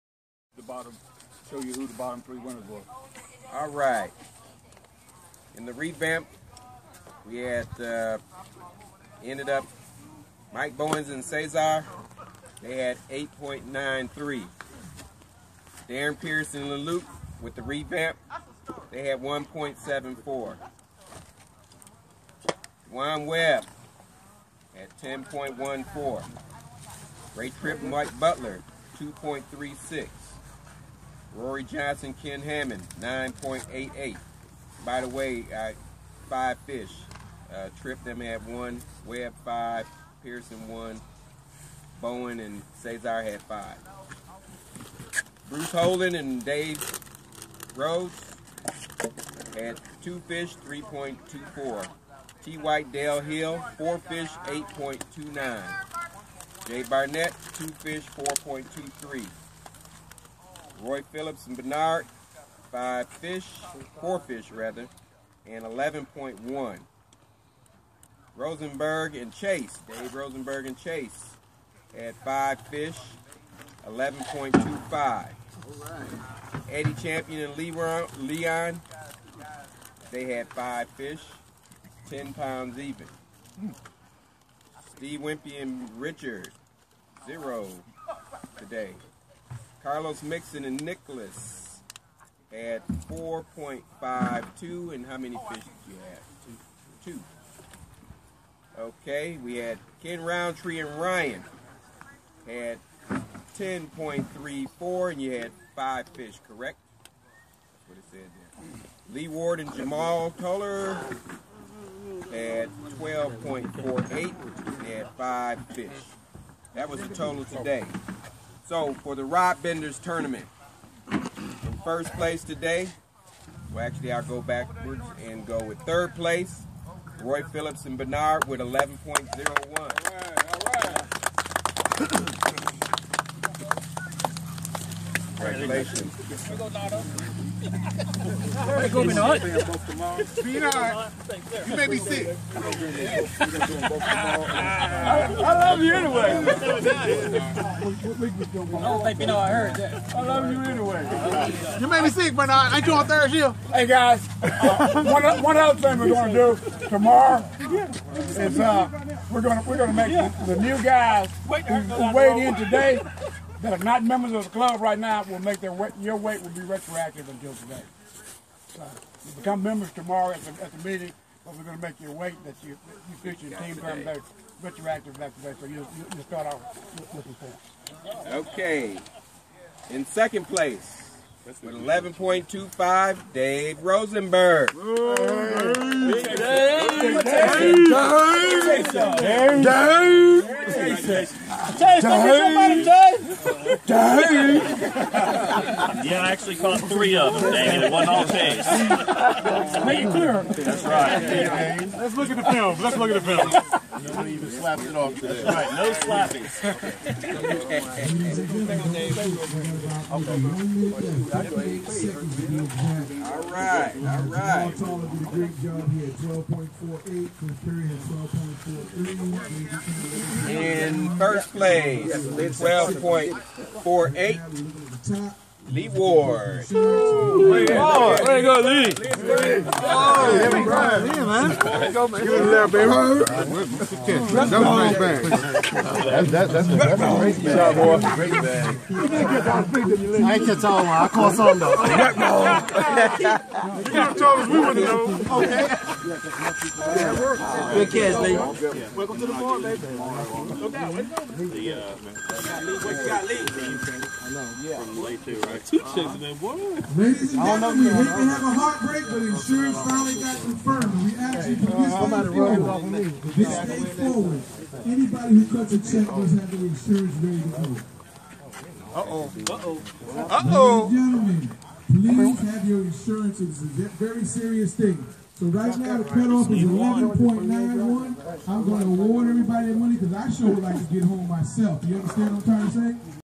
Rod Benders Bass Club tournament Lake Lanier
Rod Rod Benders Bass Club tournament Lake Lanier. 16 teams participated.